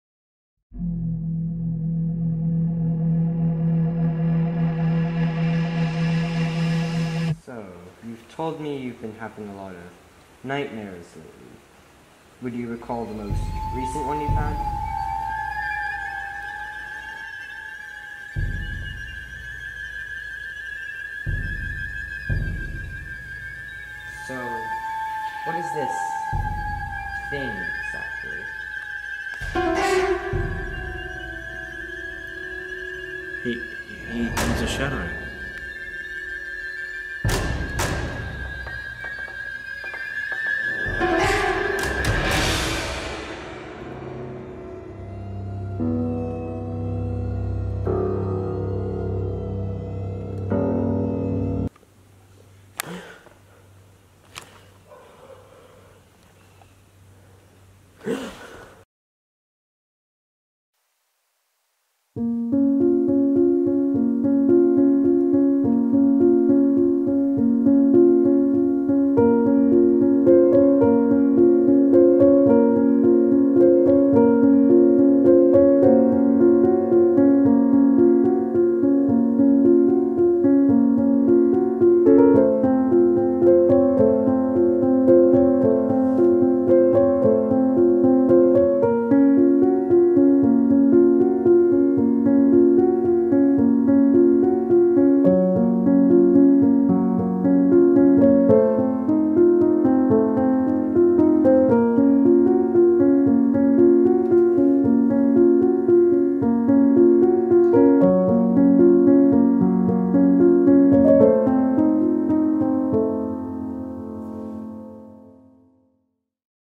Dream Therapy | Horror Shortfilm | The Theraverse
A therapy patient discusses a recent nightmare...

Written and Directed by Andrew Draper
Edited by Andrew Draper

Produced by
Darren Draper
Andrew Draper
Jane Draper

Cast
Andrew Draper
Alex Field
Truman Beach

Crew
Cinematographer:  Andrew Draper
Camera Operator:  Andrew Draper, Alex Field, Truman Beach, Darren Draper